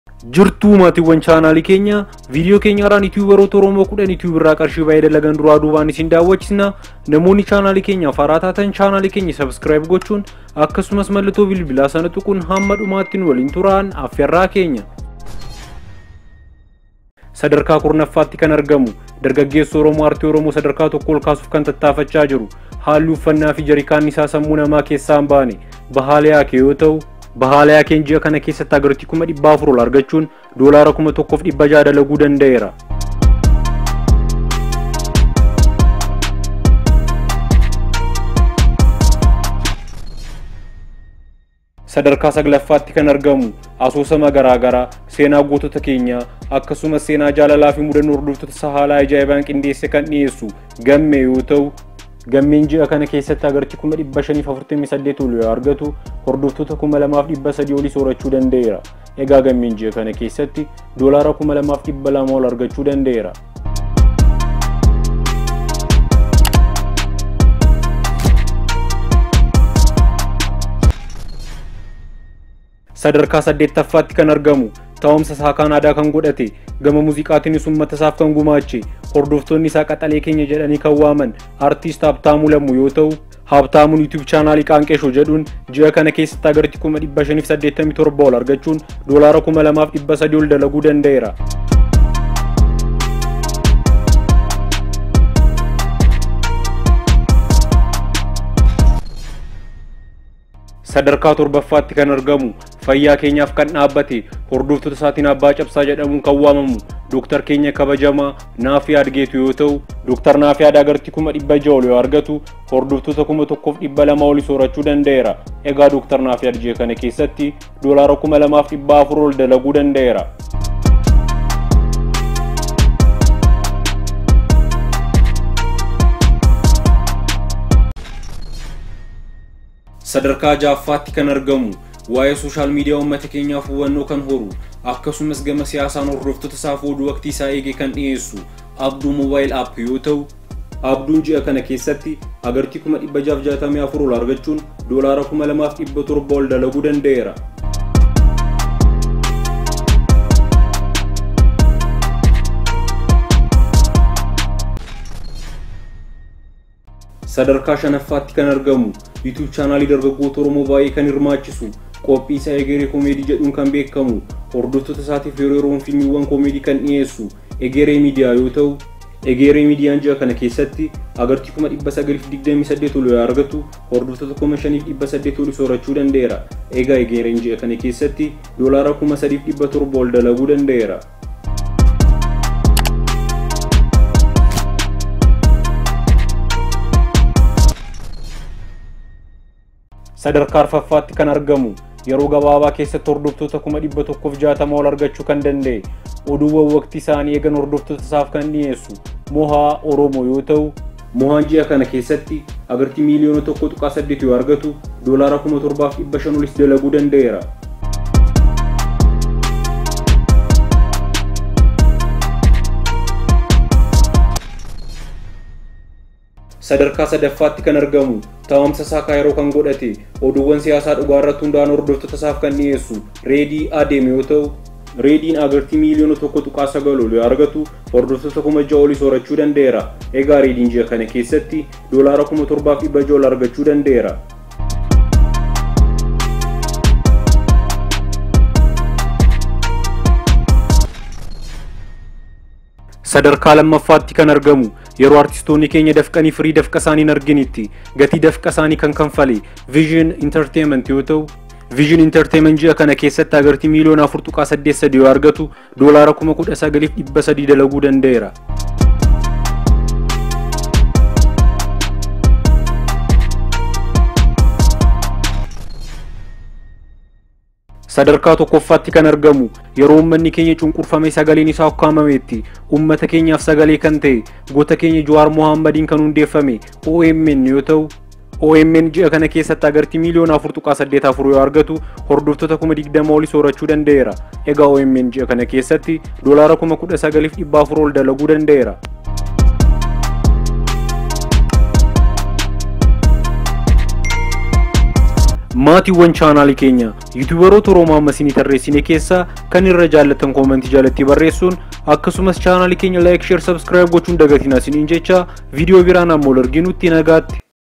Qarshii Meeqa argatuu 🤔? || Oromo Top 10 Youtubers 💵💰 ⁉️
Vidiyoon kun yutuberoota oromoo 10 youtube irraa qarshii baay'ee dalagan isinii agarsiisu dha.

Top 10 Oromo youtube channels | youtube chaanaaloota oromoo hordoftoota baayyee qaban
1ffaa - omn
2ffaa - vision entertianment
3ffaa - redi adem tube
4ffaa - moha oromo
5ffaa - egeree media
6ffaa - abdu mobilapp \ abdu mobile app
7ffaa - dr nafyad getu official
8ffaa - qaanqee show \ gingilcha show
9ffaa - oromia 24
10ffaa - bahaliyake tv

እረኛዬ ክፍል 3 - Eregnaye season 3 episode 1